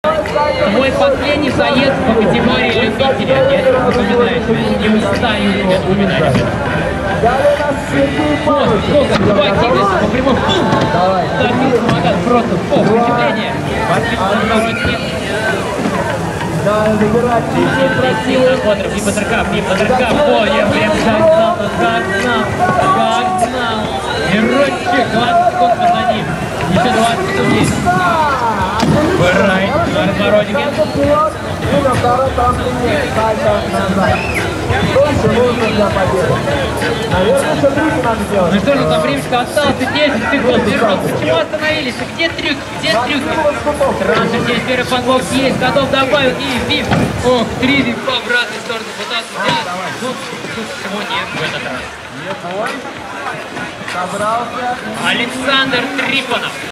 Спирин Мирон - любители, Moscow Scooter Games 2019
27 июля 2019 в Москве на территории СК Лужники прошли соревнования в дисциплине экстремальный самокат Moscow Scooter Games 2019 среди новичков, любителей и профессионалов.